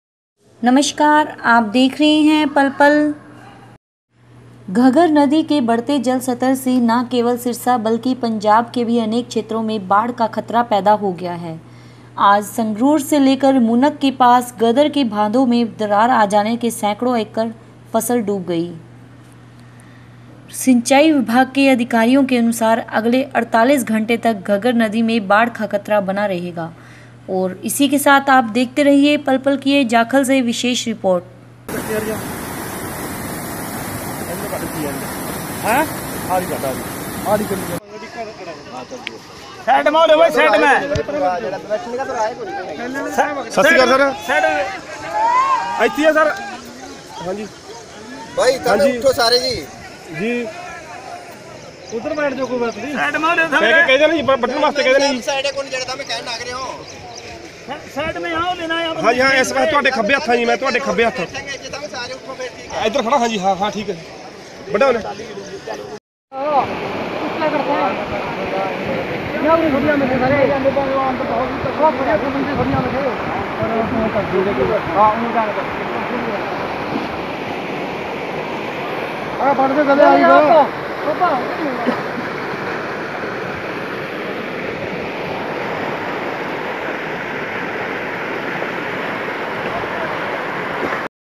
सिंचाई विभाग के अधिकारियों के अनुसार अगले 48 घंटे तक घग्गर नदी में बाढ़ का खतरा बना रहेगा
REPORT BY DEEPAKJAKHAL
150 फीट गहरे घग्गर में खलल के कारण लोगों में हडकंप मचा हुआ है।  फिर भी, दरार अभी तक नहीं बनी है।  कई गांवों में हजारों एकड़ फसलें भी पानी की चपेट में आ गई हैं।  राज्य में गुरुवार सुबह भारी बारिश के कारण संगरूर जिले के खुनोरी और मुनक इलाकों से गुजरने वाली घग्गर नदी का जल स्तर खतरे के निशान से ऊपर पहुंच गया।  लगभग 8 घंटे के बाद, जिले के उपायुक्त घनश्याम थोटी ने सेना की व्यवस्था की और एन।  डी  आर ..  F से मदद मांगी।  उल्लेखनीय है कि यहां बारिश के कारण 30 हजार से अधिक घर जलमग्न हो गए हैं।  इन घरों की सामग्री पानी में खराब हो गई है।
 शहर के अलावा, ग्रामीण इलाकों की स्थिति भी काफी गंभीर रही है।  कई गाँवों में बने तालाब ओवरफ्लो हो गए हैं और उनका पानी गाँवों में घुस गया है।  जिले की अधिकांश बाढ़ जैसी स्थिति अभी भी बनी हुई है।  ज्यादातर इलाकों में 3 से 4 फीट तक पानी भर गया है और कई इलाकों में लोग अपने घरों में कैद हो गए हैं।  यह उल्लेख किया जाना है कि यदि टूटी हुई दीवार जल्द ही पूरी नहीं हुई, तो आसपास के गांवों में बाढ़ आ जाएगी, जिससे लोग अपने घरों में फंस सकते हैं।  मौके पर लोगों ने कहा कि टूटे हुए हिस्सों के कारण उनकी फसल को काफी नुकसान हो रहा है, जिसके कारण वे इसे बंद करने की कोशिश कर रहे हैं।